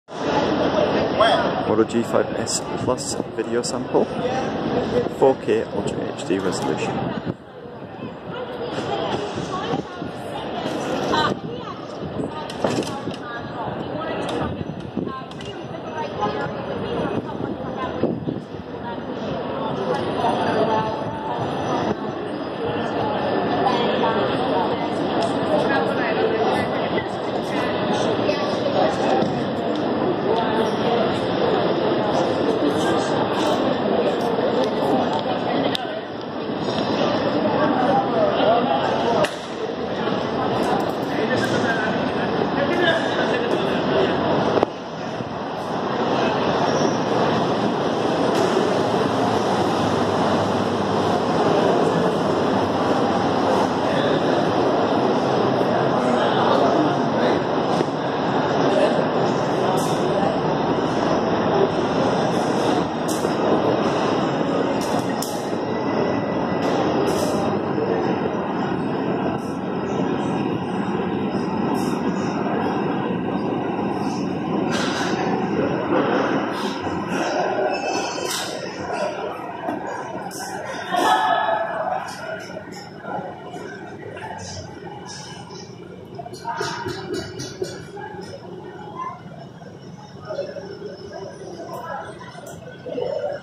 Moto G5s Plus Camera Test: 4K video sample
To test the Motorola Moto G5s Plus' dual-lens camera, we shot this 4K video sample in Covent Garden.

The Moto G5s Plus sports a new 13-megapixel dual-lens camera which can shoot up to 4K resolution video, or Full HD footage at 30 or 60 FPS. Here we test the G5s Plus' camera with some 4K UHD video recording in central London.

Check back soon for our full Moto G5s Plus camera review and comparison with the single-lens camera of the G5 Plus.